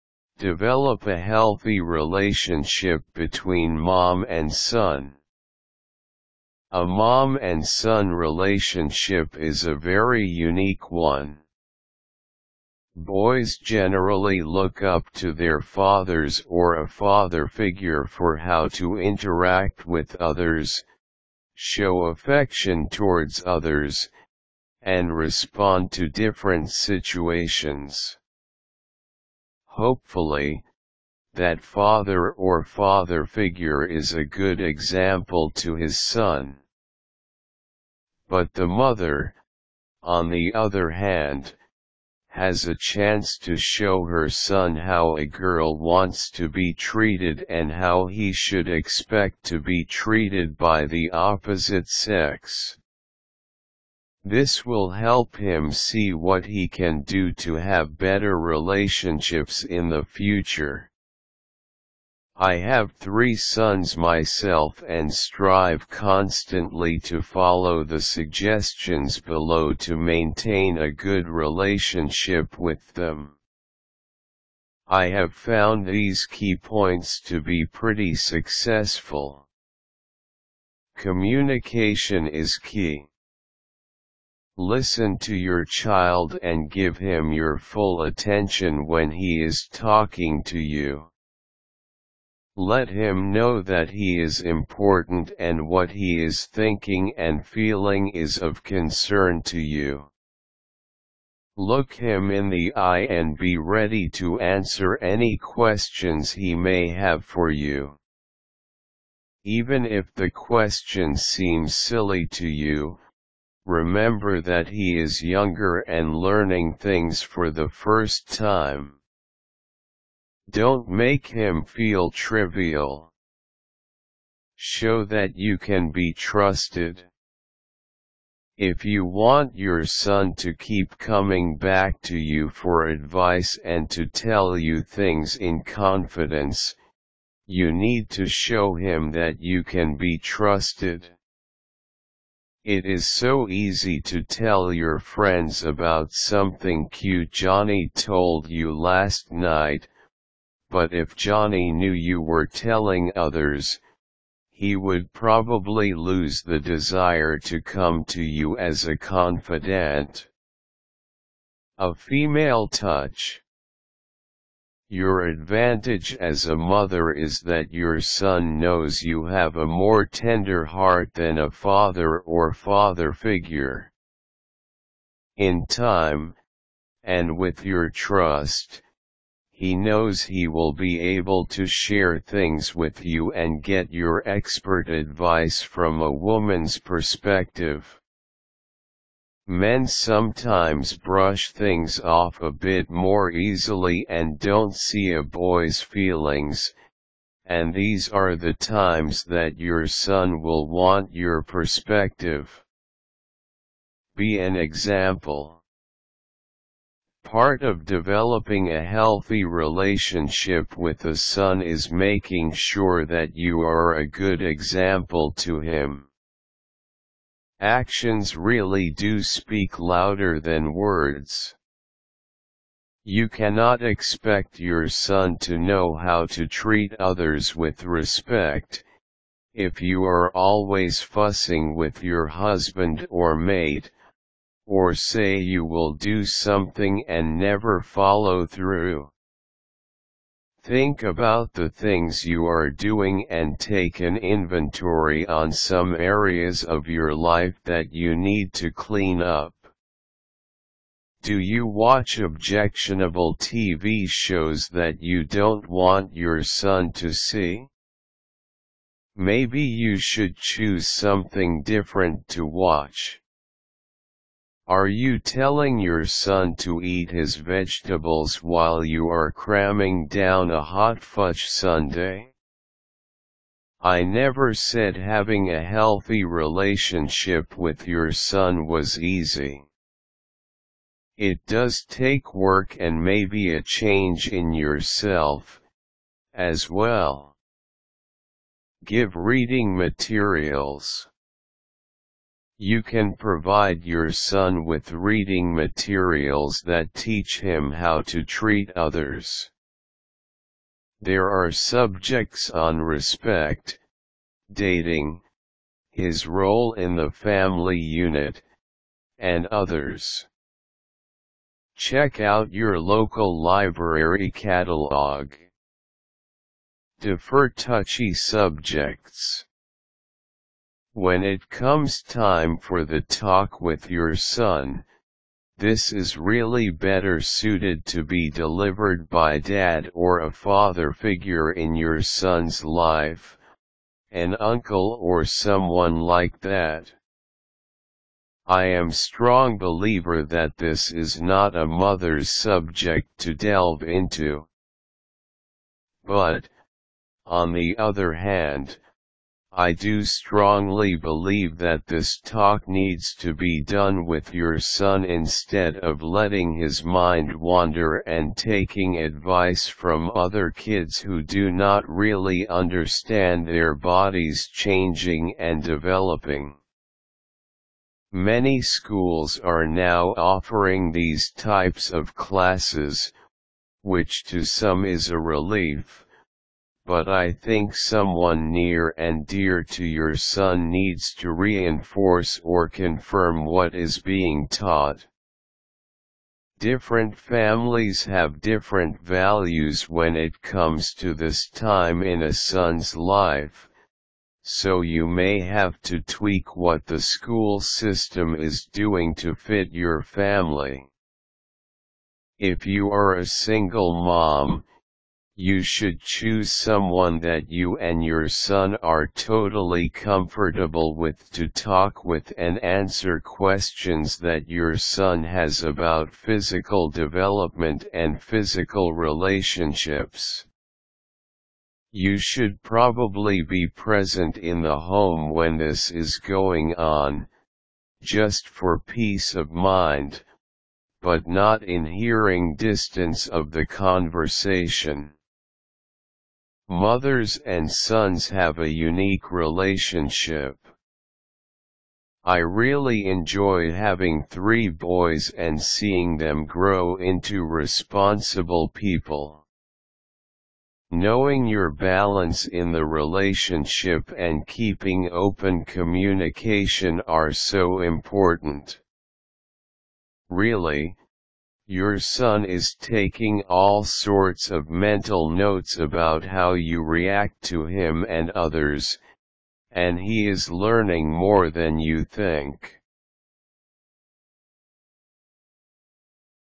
How to : Develop a healthy relationship between mom and son
Develop a healthy relationship between mom and son

 A mom and son relationship is a very unique one. Boys generally look up to their fathers or a father-figure for how to interact with others, show affection towards others, and respond to different situations. Hopefully, that father or father-figure is a good example to his son. But the mother, on the other hand, has a chance to show her son how a girl wants to be treated and how he should expect to be treated by the opposite sex. This will help him see what he can do to have better relationships in the future. I have three sons myself and strive constantly to follow the suggestions below to maintain a good relationship with them. I have found these key points to be pretty successful.

 Communication is key . Listen to your child and give him your full attention when he is talking to you. Let him know that he is important and what he is thinking and feeling is of concern to you. Look him in the eye and be ready to answer any questions he may have for you. Even if the question seems silly to you, remember that he is younger and learning things for the first time. Don't make him feel trivial. Show that you can be trusted . If you want your son to keep coming back to you for advice and to tell you things in confidence, you need to show him that you can be trusted. It is so easy to tell your friends about something "cute" Johnny told you last night, but if Johnny knew you were telling others, he would probably lose the desire to come to you as a confidant. A female touch . Your advantage as a mother is that your son knows you have a more tender heart than a father or father-figure. In time, and with your trust, he knows he will be able to share things with you and get your expert advice from a woman's perspective. Men sometimes brush things off a bit more easily and don't see a boy's feelings, and these are the times that your son will want your perspective. Be an example . Part of developing a healthy relationship with a son is making sure that you are a good example to him. Actions really do speak louder than words. You cannot expect your son to know how to treat others with respect, if you are always fussing with your husband or mate, or say you will do something and never follow through. Think about the things you are doing and take an inventory on some areas of your life that you need to clean up. Do you watch objectionable TV shows that you don't want your son to see? Maybe you should choose something different to watch. Are you telling your son to eat his vegetables while you are cramming down a hot fudge sundae? I never said having a healthy relationship with your son was easy. It does take work and maybe a change in yourself, as well. Give reading materials . You can provide your son with reading materials that teach him how to treat others. There are subjects on respect, dating, his role in the family unit, and others. Check out your local library catalog. Defer touchy subjects . When it comes time for the "talk" with your son, this is really better suited to be delivered by dad or a father-figure in your son's life (an uncle or someone like that). I am strong believer that this is not a mother's subject to delve into. But, on the other hand, I do strongly believe that this talk needs to be done with your son instead of letting his mind wander and taking advice from other kids who do not really understand their bodies changing and developing. Many schools are now offering these types of classes, which to some is a relief, but I think someone near and dear to your son needs to reinforce or confirm what is being taught. Different families have different values when it comes to this time in a son's life, so you may have to tweak what the school system is doing to fit your family.

 If you are a single mom, you should choose someone that you and your son are totally comfortable with to talk with and answer questions that your son has about physical development and physical relationships. You should probably be present in the home when this is going on, just for piece of mind, but not in hearing distance of the conversation.

 Mothers and sons have a unique relationship. I really enjoy having three boys and seeing them grow into responsible people. Knowing your balance in the relationship and keeping open communication are so important. Really, your son is taking all sorts of mental notes about how you react to him and others, and he is learning more than you think!